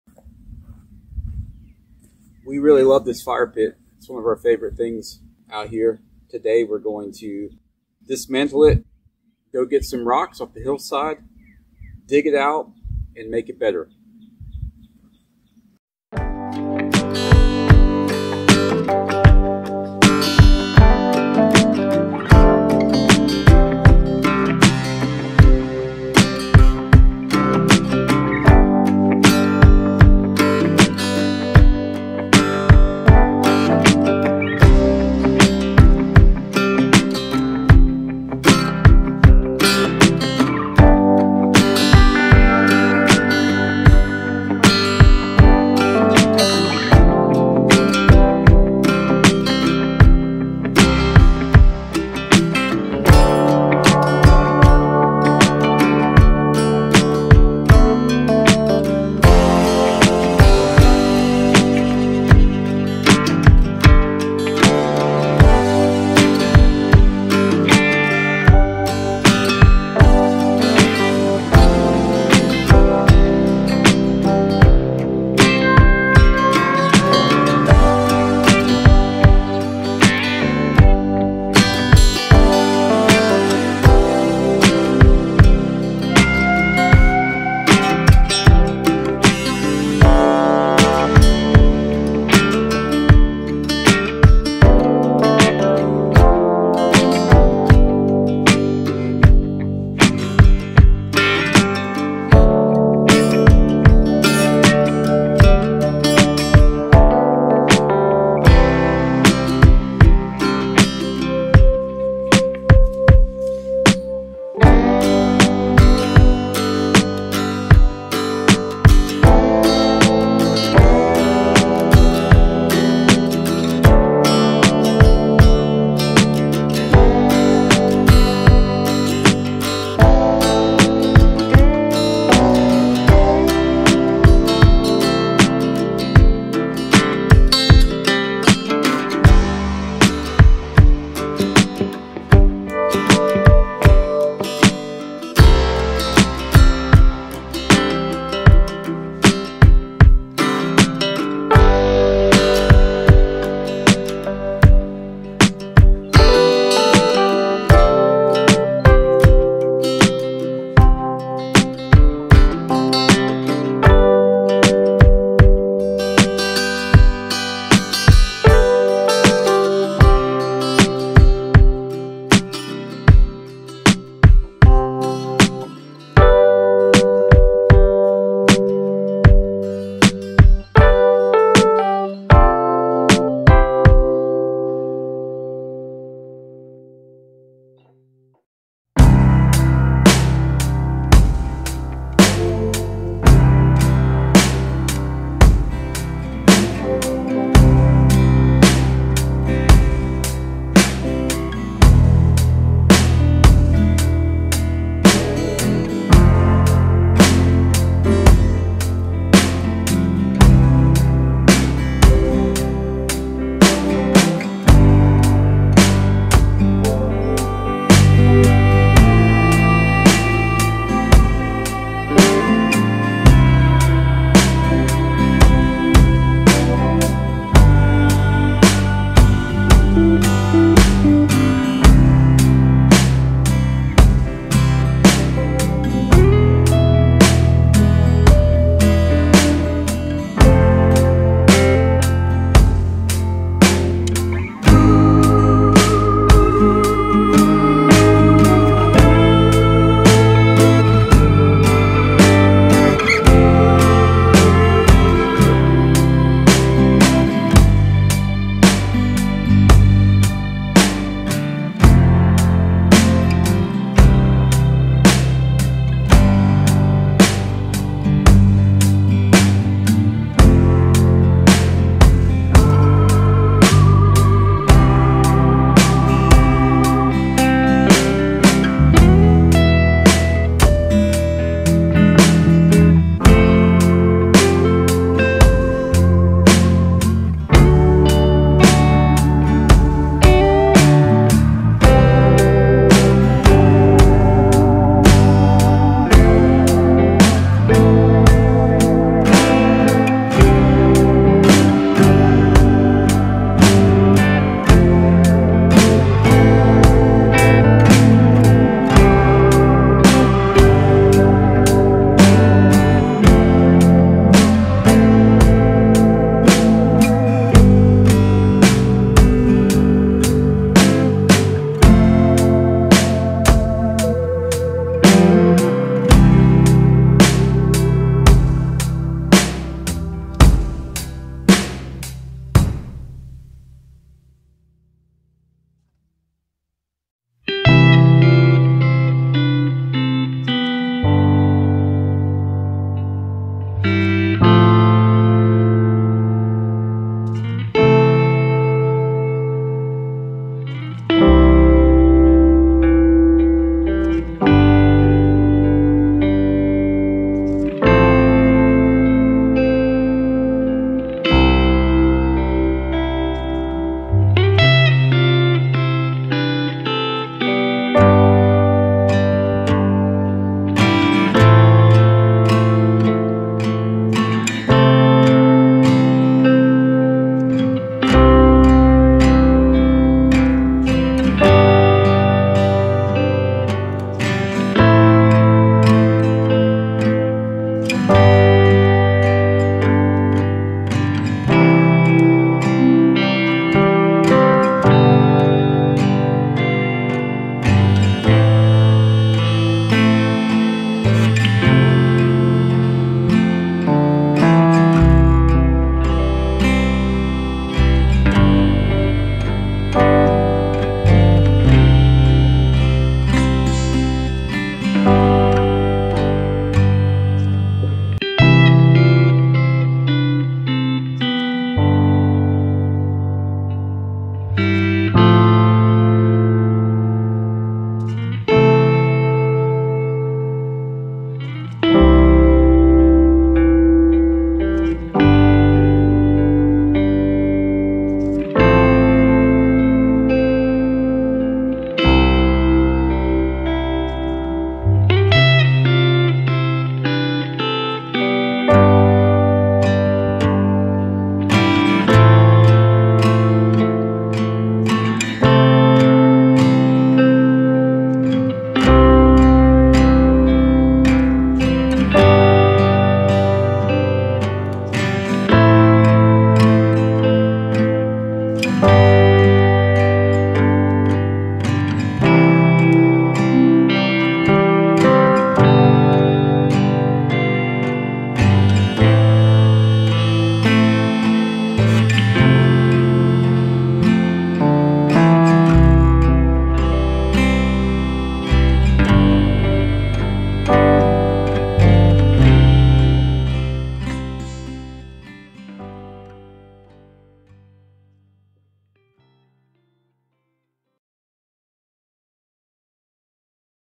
Building a Natural Rock Fire Pit - Our Favorite Design for a Firepit
The old firepit was just a ring of rocks and had become full of ashes over the past few years. It was no longer safe and wasn't really nice to look at. We take apart the old firepit, find some rocks in the river and on the mountain, and put it all back together, bigger and more functional! The new firepit is aesthetically pleasing, safe, and funnels heat and smoke appropriately. We've looked at a lot of firepits in the area, and this is our favorite design.